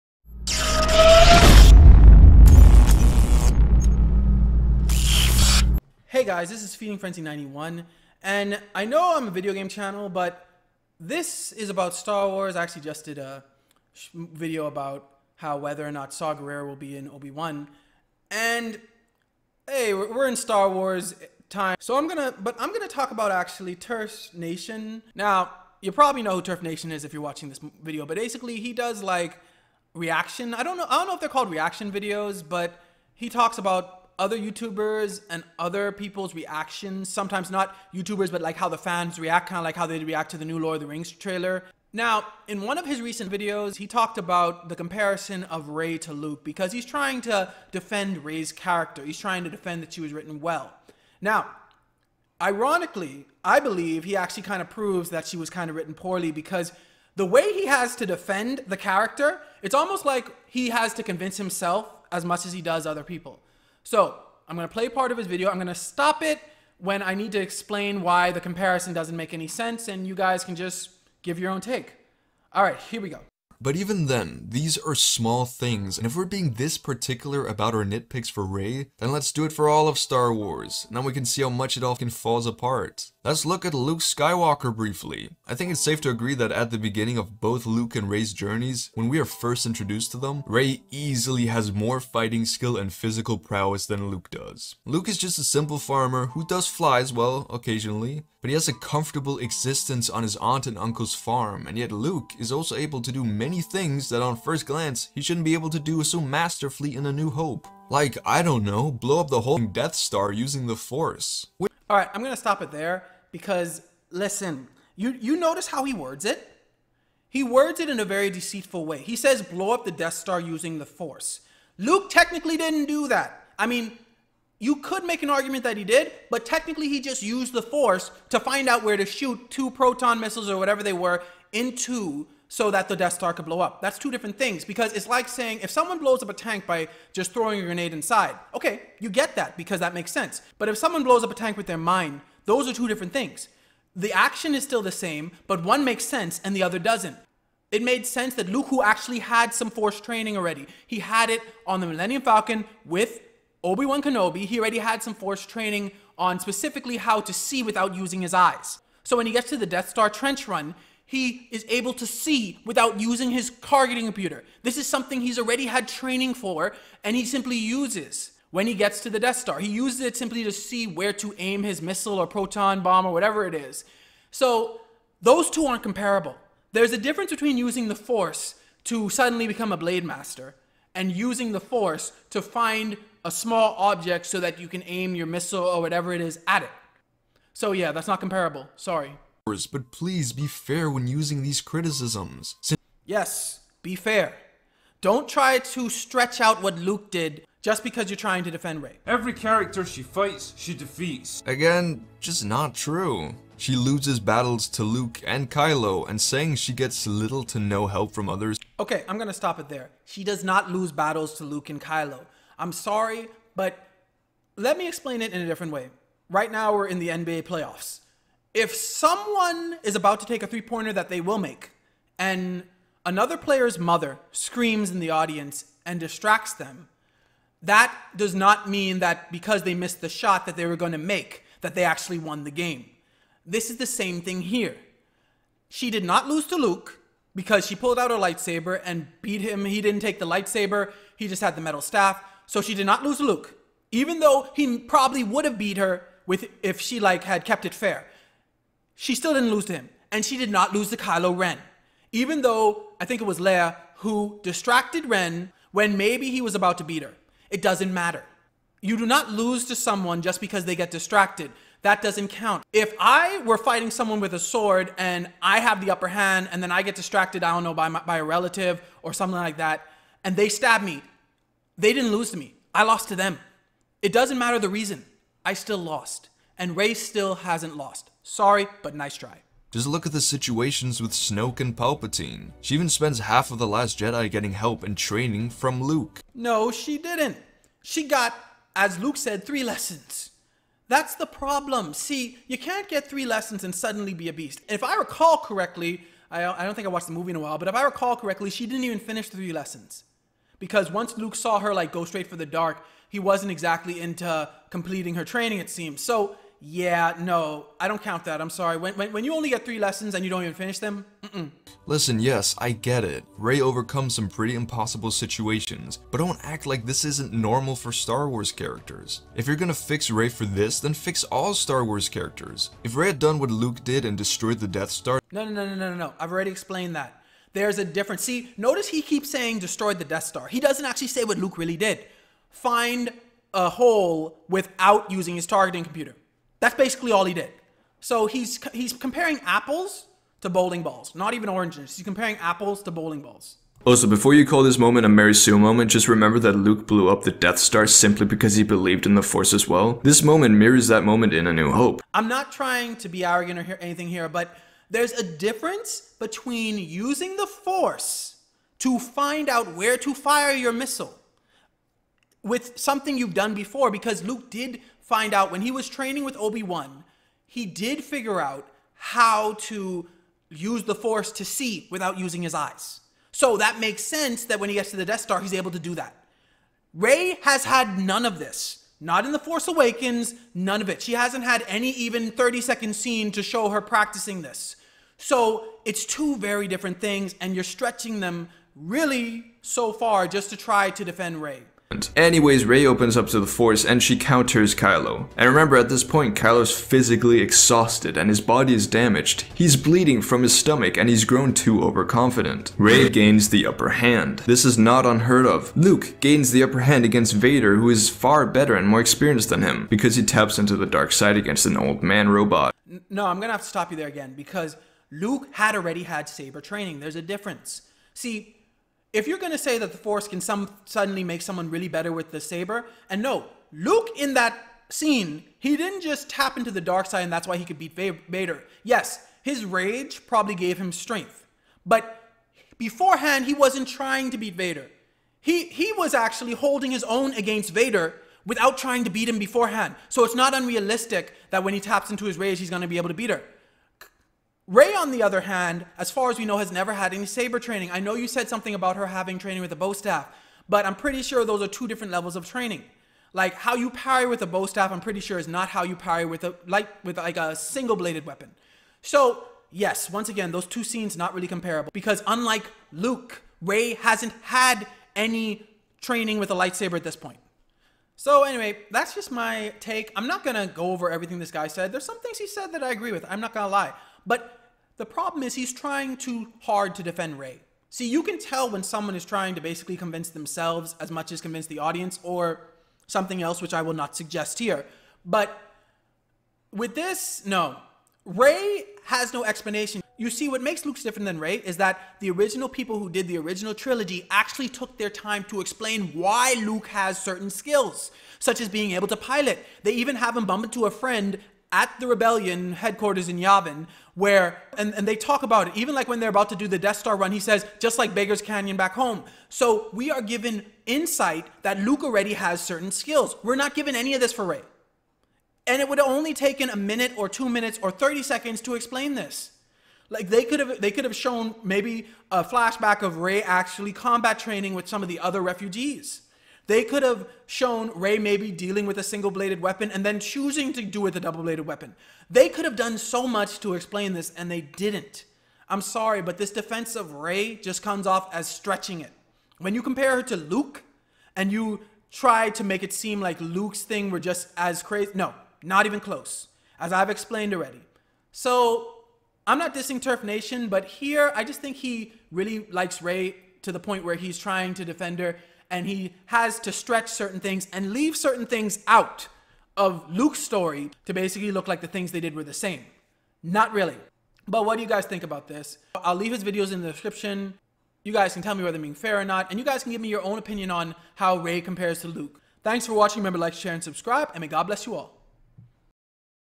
Turfnation compares Rey to Luke (And does a terrible job)
Nice try, but the stretching is real with this comparison.